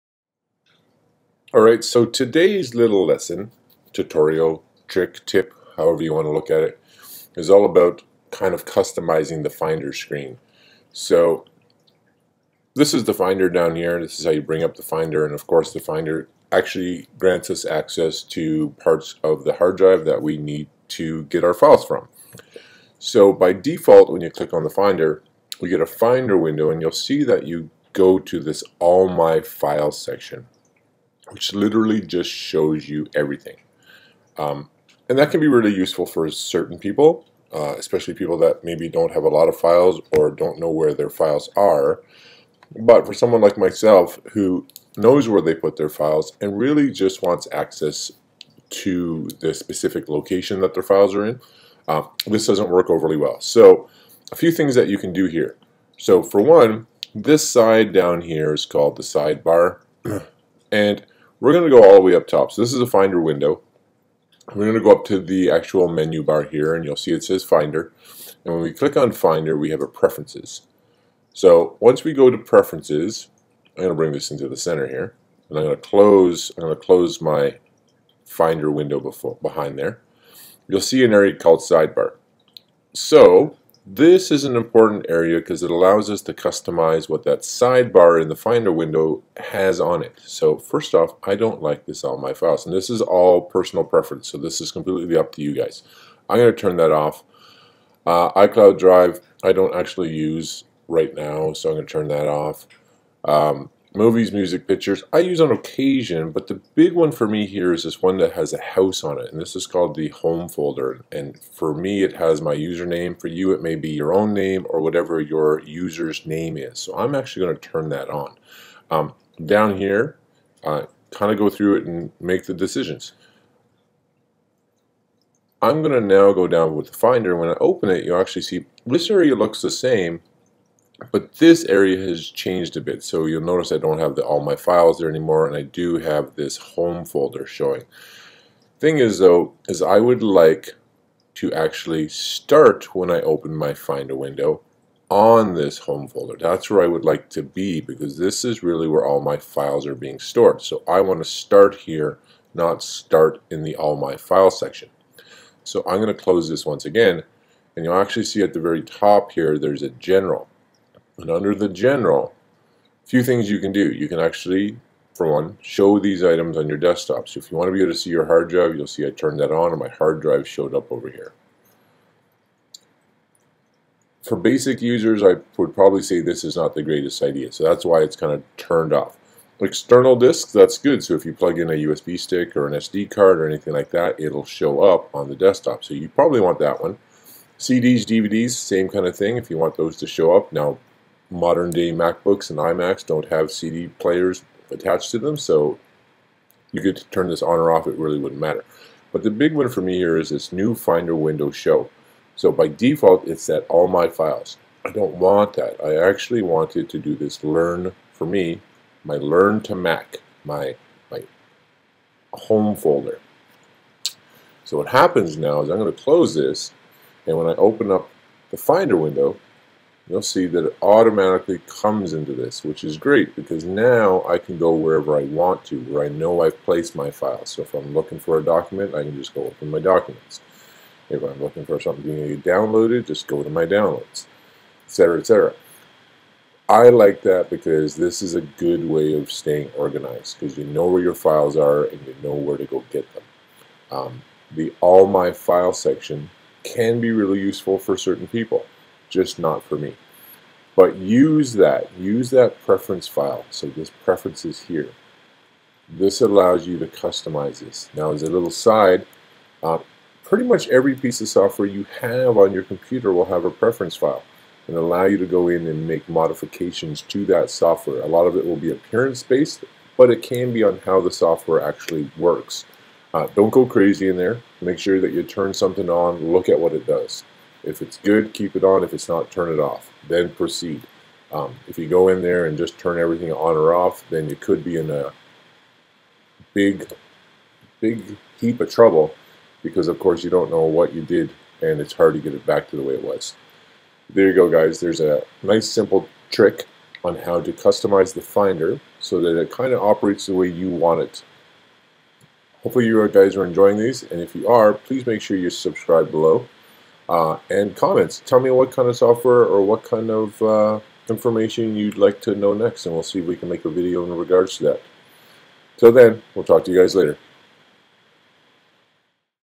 Learn To Mac - How to Customize the Mac's Finder Tutorial
we take a look at how to customize the mac's finder screen.  so that it shows exactly what we want it to show.